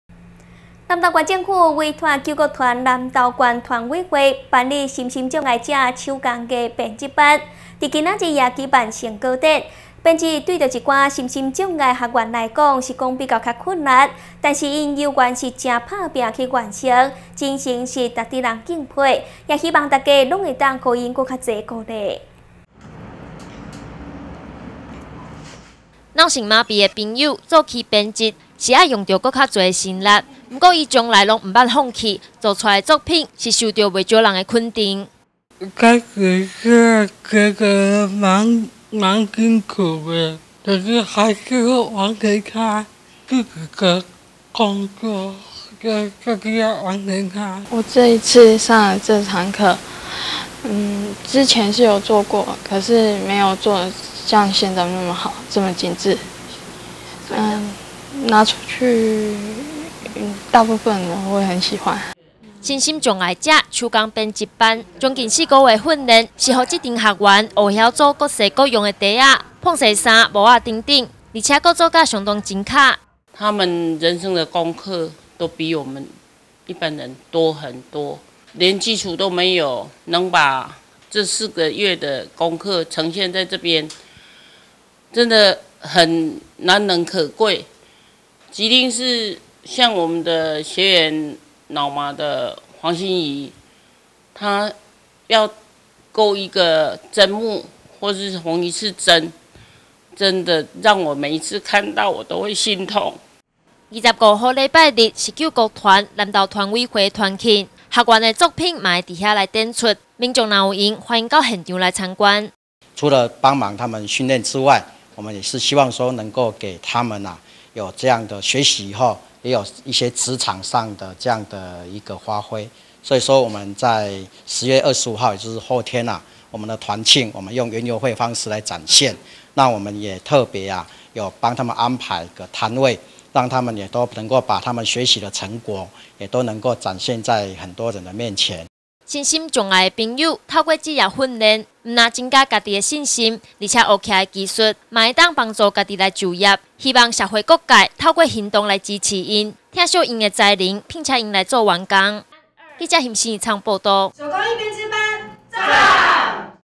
南投新聞-南投身心障礙者手工藝編織班成果展
南投縣政府，委託救國團南投縣團委會，辦理的身心障礙者手工藝編織班，在今日舉辦成果展，編織對一些身心障礙的學員來說，是比較困難，但他們仍然努力去完成，精神令人相當敬佩，也希望大家都可以給予他們更多的鼓勵。